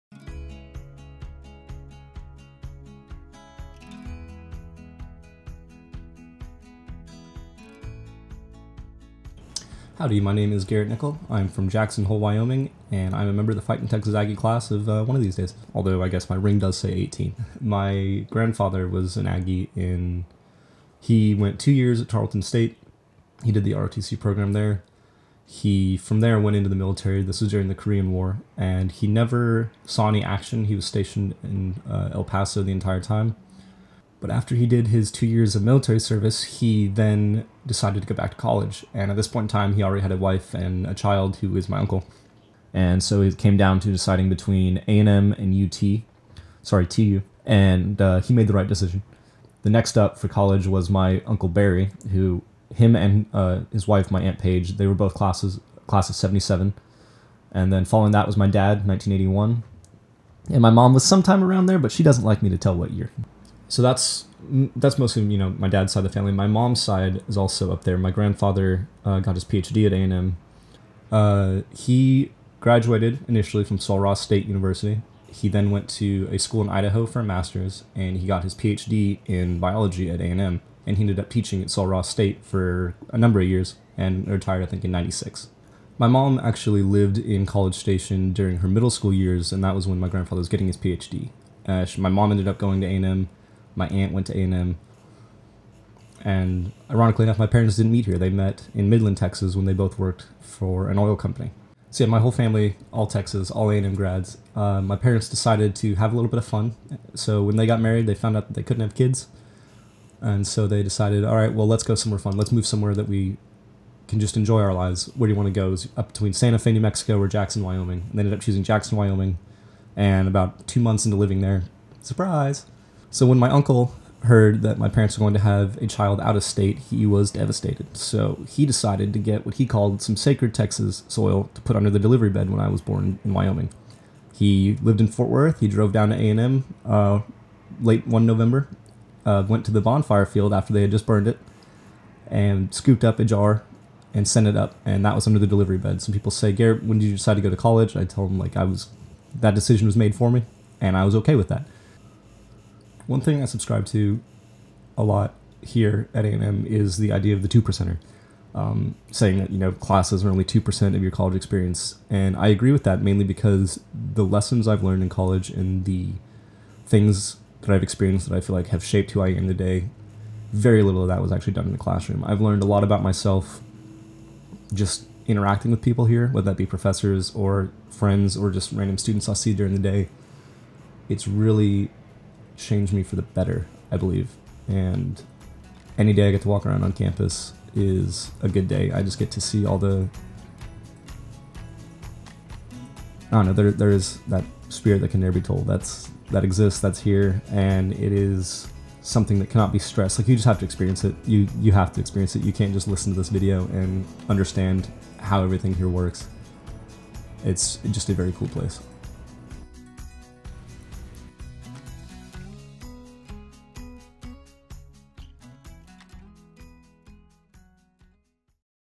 My Aggie Story: Garrett Nickell
Born in Wyoming over bonfire field soil. Both parents are Aggies with a long line on both sides.